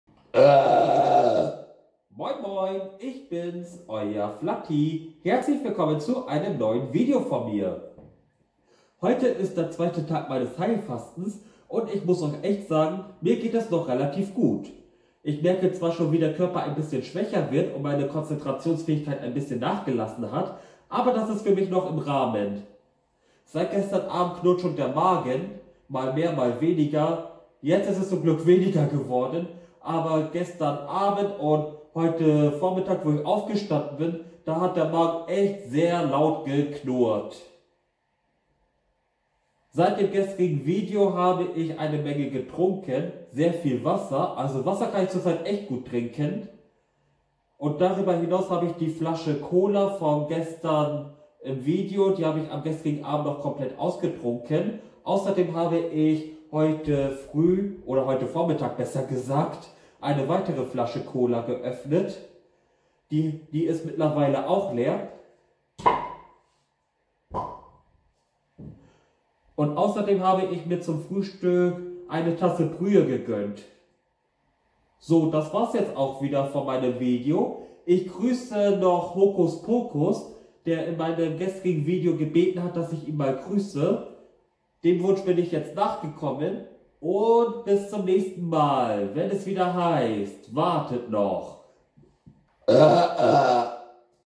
Heilfasten - Tag 2
Am 13. März 2020 habe ich mit dem Heilfasten angefangen. In dieser Zeit werde ich jeden Tag ein kurzes Video veröffentlichen, wo ich erzähle, wie es mir dabei so geht.